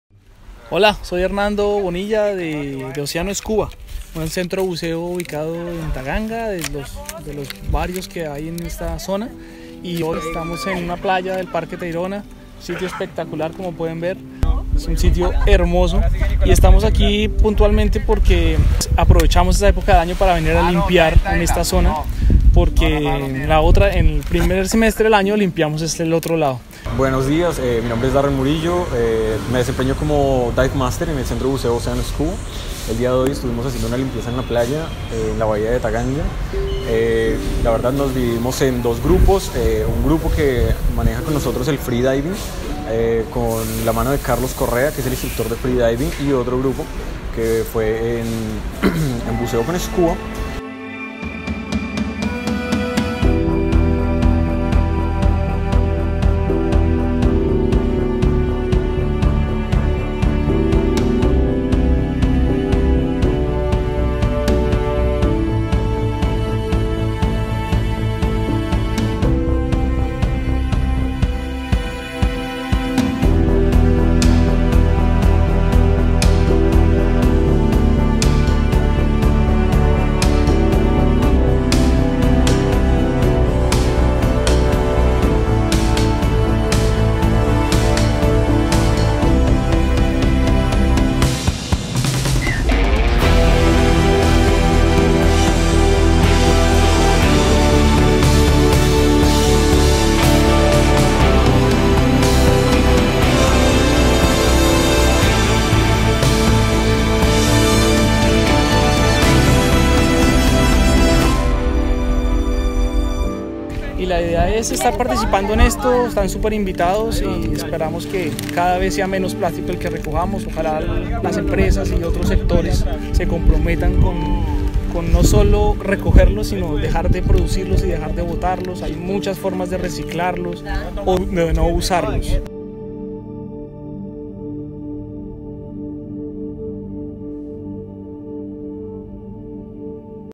How far would you go to clean up the oceans?
This year we happened to be in Colombia for the International beach clean up day and we were lucky enough to meet the people from Oceano Scuba in Taganga.
It was amazing! This guys are incredible, they started cleaning a few days earlier some remote beaches. We cleaned the beach but also we went free diving picking up rubbish underwater and even a team of scuba divers went in to do the same.